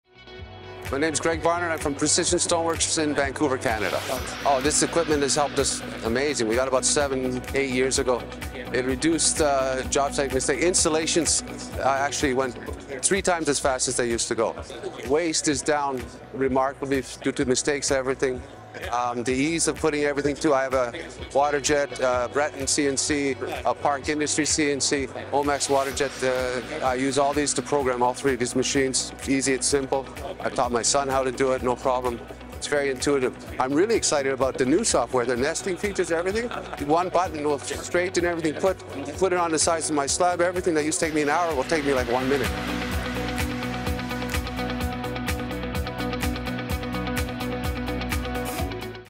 Greg Barnert discusses the LT-2D3D and LPI's new LT3Raptor software
Greg Barnert of Precision Stone Works Ltd. in Vancouver, BC Canada discusses how Going Digital with LPI and the LT-2D3D has made his company faster and more efficient, including the intuitive design of the software making it easy to learn, operate and interact with his CNC machines. After seeing a demo of the new LT3Raptor software, he also discusses what features he's excited about that will make his team even quicker. LT3Raptor GoDigital with LPI

and sign up to not only keep up to date on the latest news on new product developments such as the LT3Raptor, but also product testing opportunities, exclusive promotions and more!